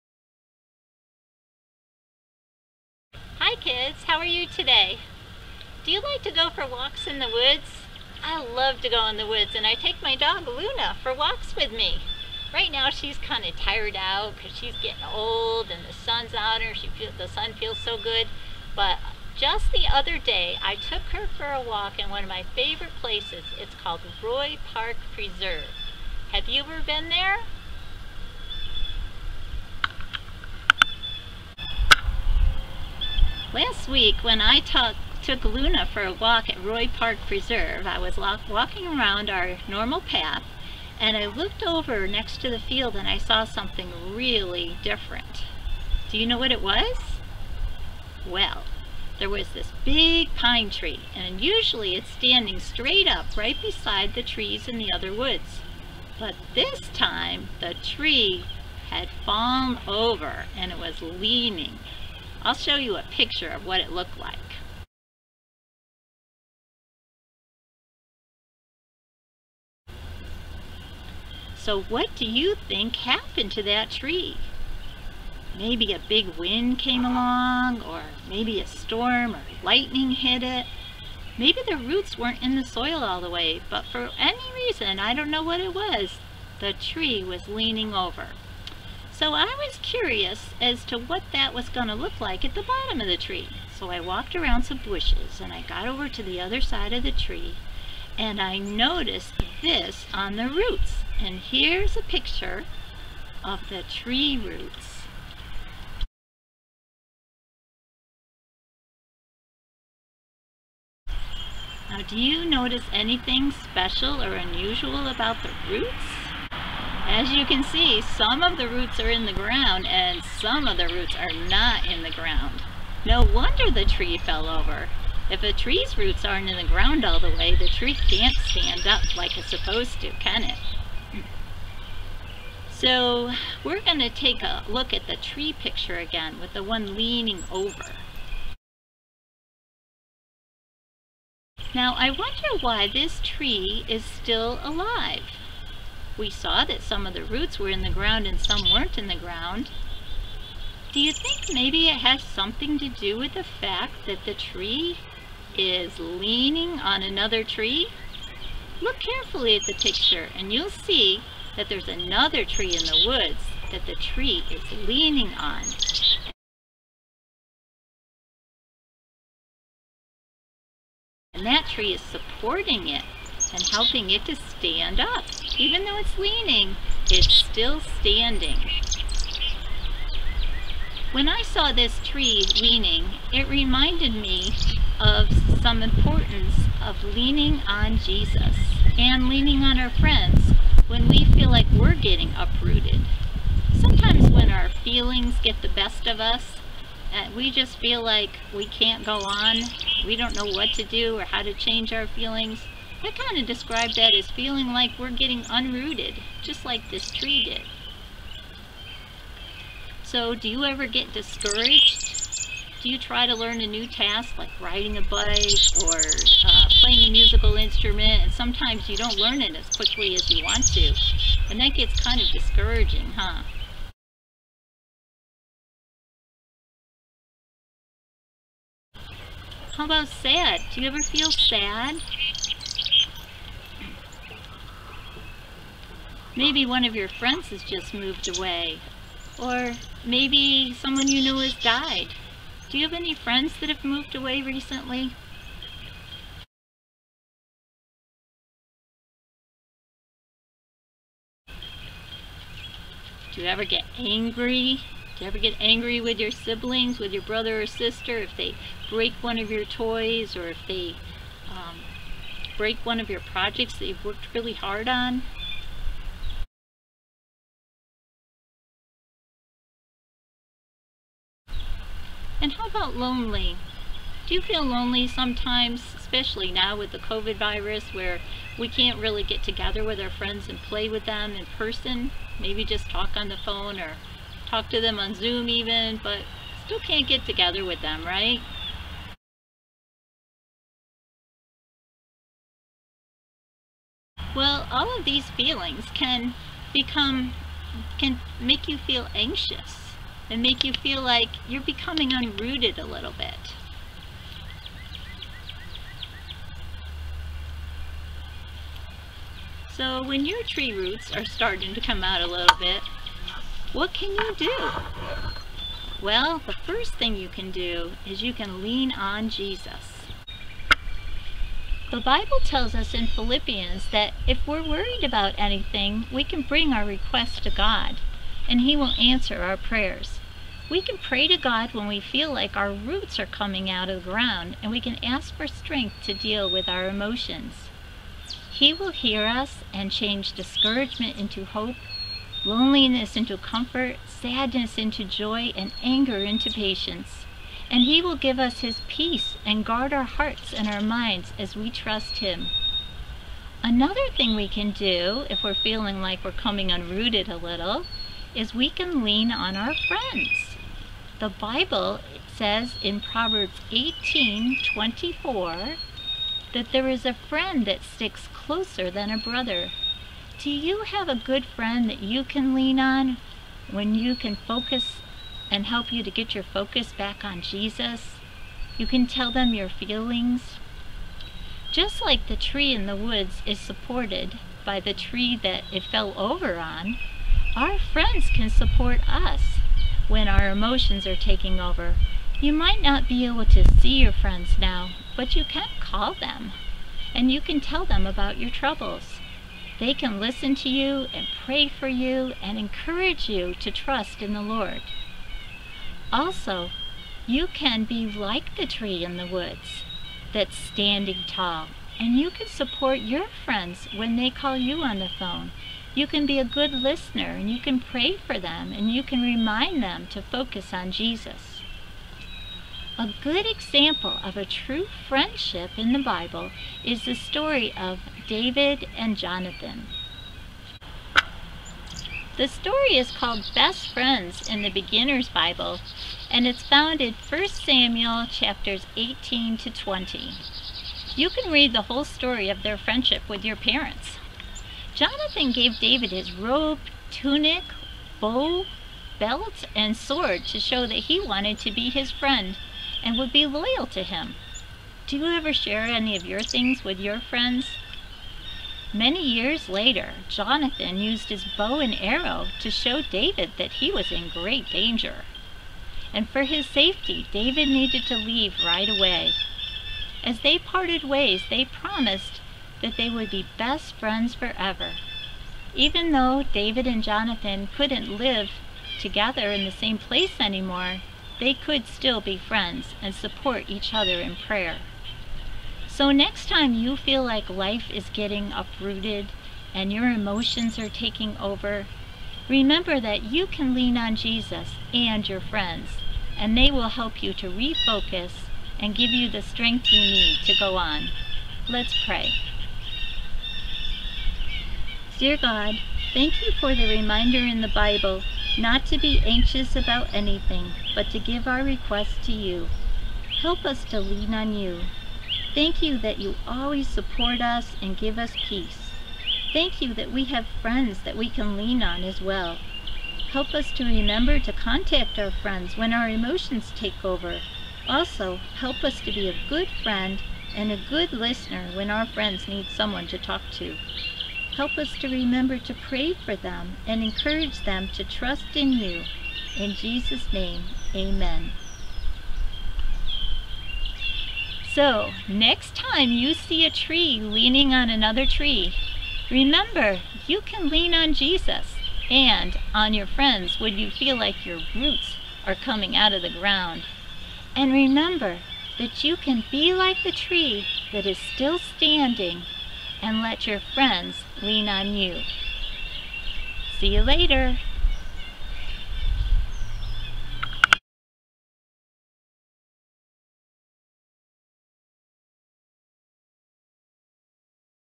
Leaning on Jesus K-5 5-21-20
Amy tells of her experience walking with her dog and discovering a tree leaning on another tree.  This reminded her of how we can lean on Jesus and our friends when we feel uprooted or when our emotions are taking over.  Amy gives examples of emotions that we might feel and how to lean on Jesus and our friends for support.